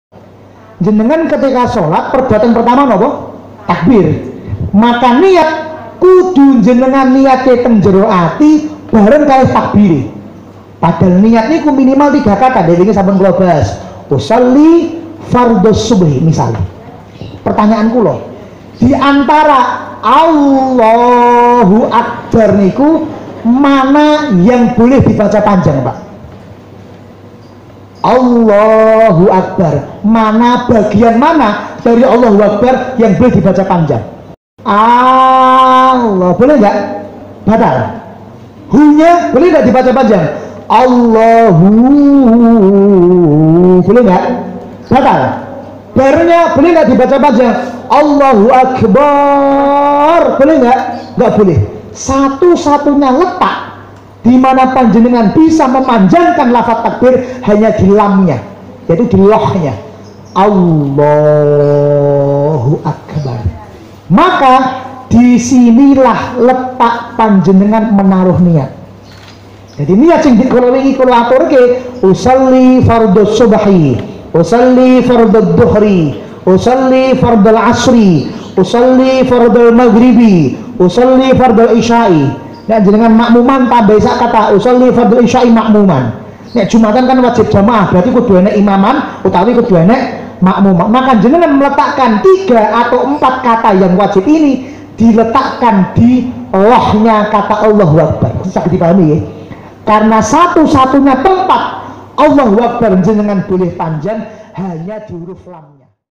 Awas !! Perhatikan !! Disinilah letak niat dalam Sholat
*AWAS!! PERHATIKAN!!*
*DISINILAH LETAK NIAT DIDALAM SHOLAT* ✨

Jadi selama ini niat sholatmu yang bagaimana ? Yakin sudah *SAH* ?

Yuk simak penjelasan singkat dari Gus Ahmad Rifai berikut ini.
Hanya 1 menit sudah mengantongi banyak ilmu. Jangan lewatkan 💚

📚 WA Grup Buya Soni
📰 Diizinkan untuk di sebarluaskan